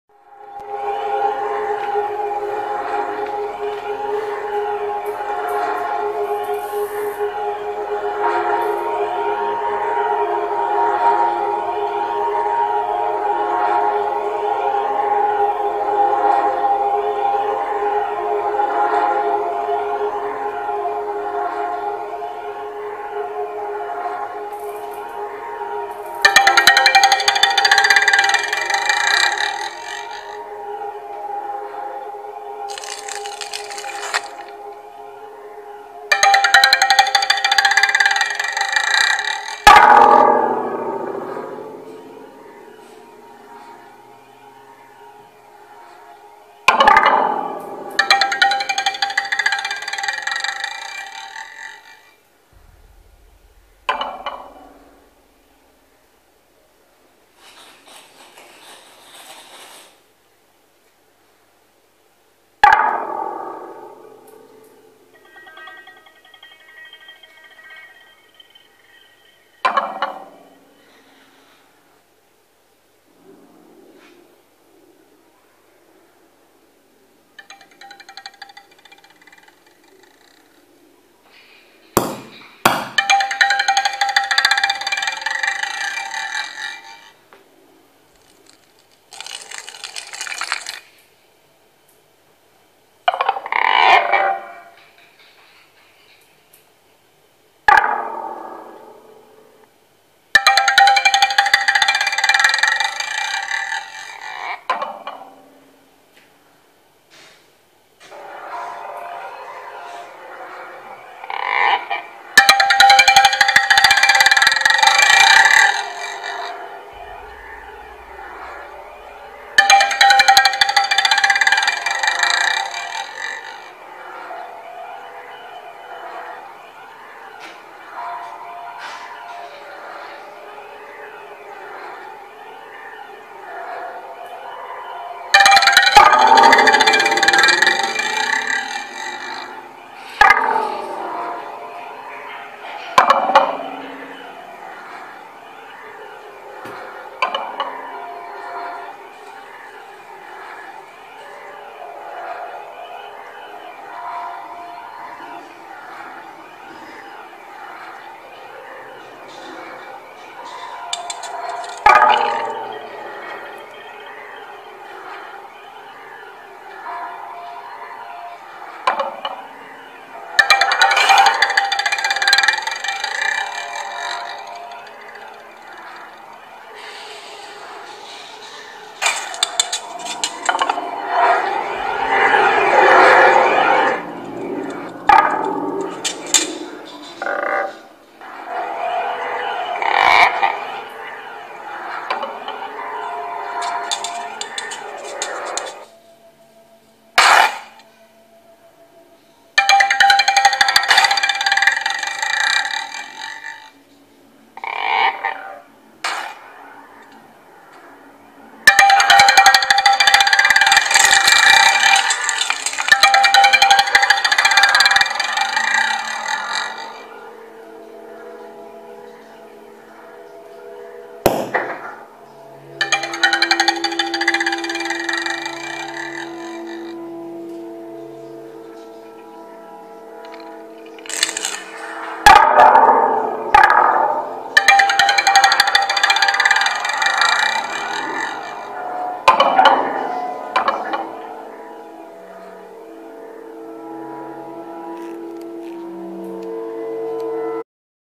De Leslies (Horst Rickels / Robert Pravda) live in Studio Loos #2
De Leslies (Horst Rickels / Robert Pravda) live in Studio Loos (Wonderwerp Series) 18 december 2014.

Horst Rickels and Robert Pravda will perform with 8 speaker-objects obtained by repurposing the Leslie speaker, which is the sounding component of the Hammond organ. After having been hidden in the cabinets of Hammond organs for decennia, now these Leslie speakers find a new life.